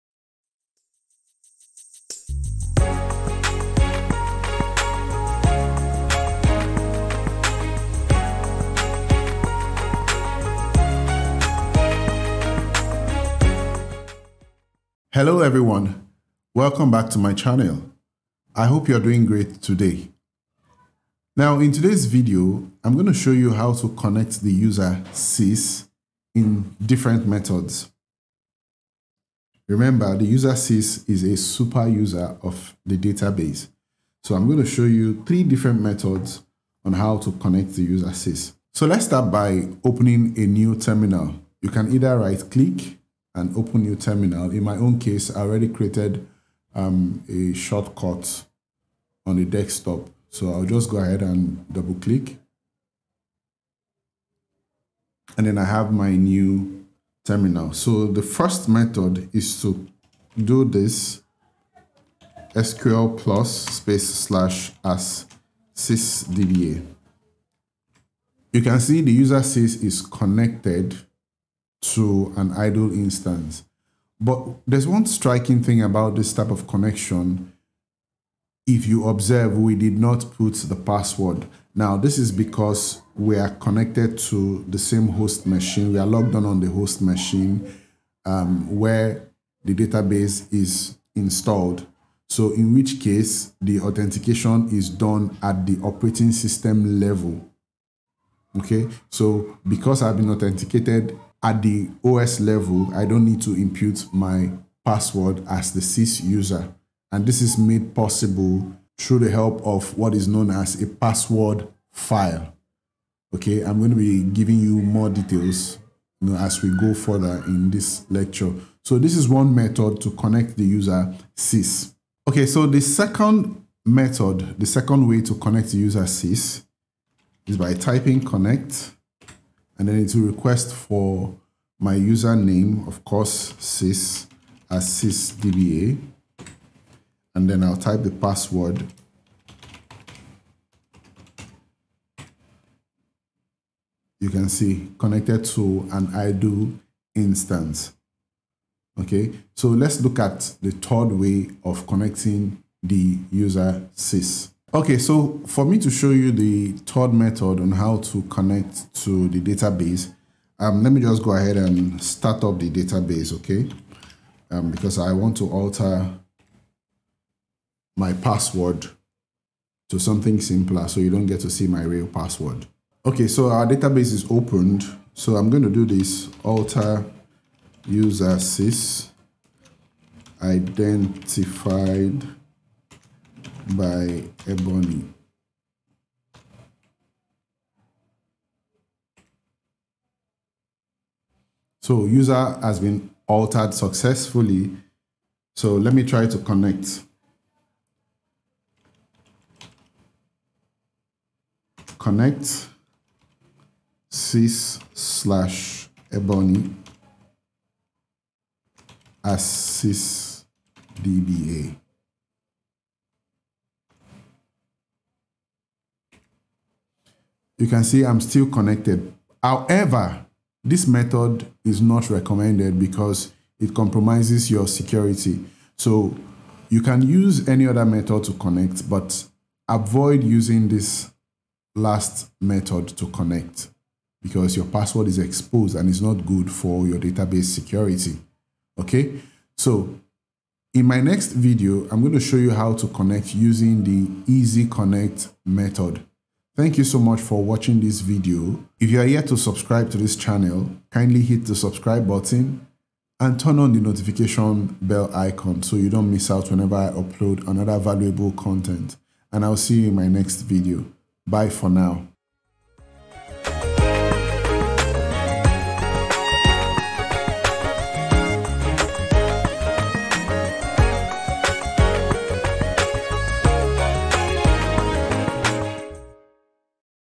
DIFFERENT METHODS TO CONNECT SYS USER
There are a lot of options available to connect the SYS user to the database. Watch this videos for more details.